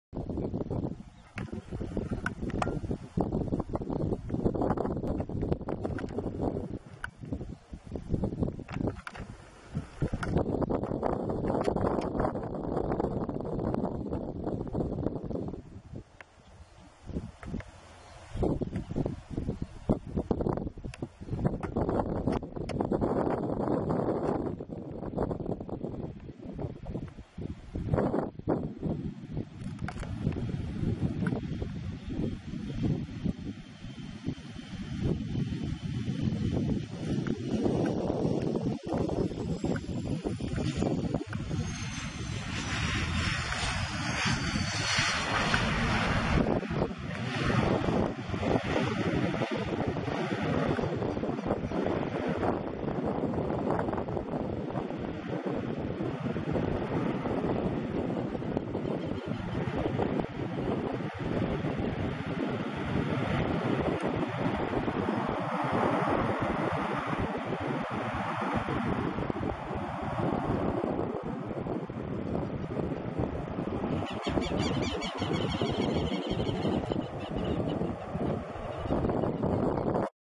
Cessna 560 Citation Ultra -LV-CTX- aterrizando en Rosario (SAAR) 7/3-15
Spotters -SAAR AIR ROS- [f]